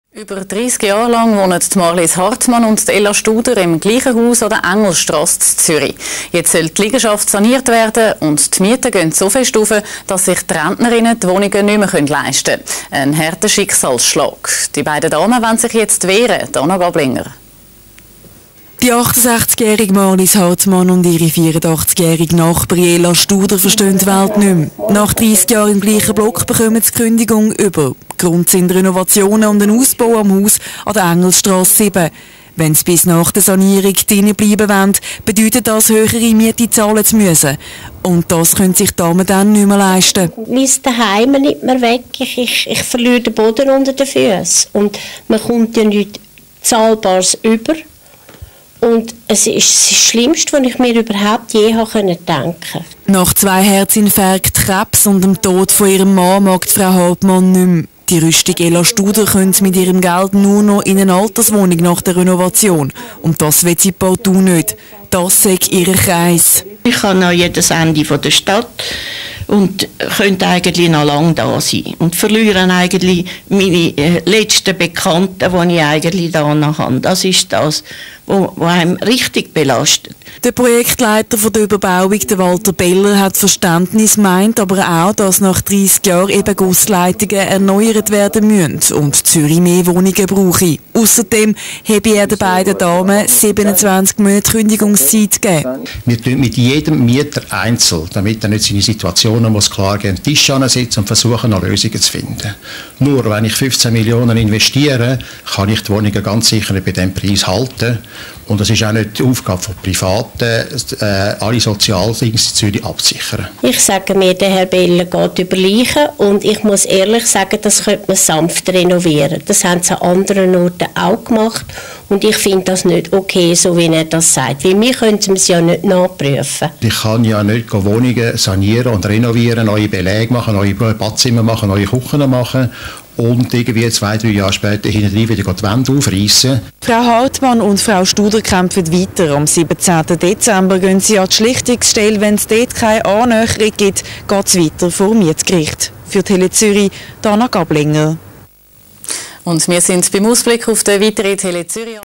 Cancellation Of Tenancy
and watch Dana Gablinger's news report from October 16, 2009 about cancellation of tenancy after 30 years.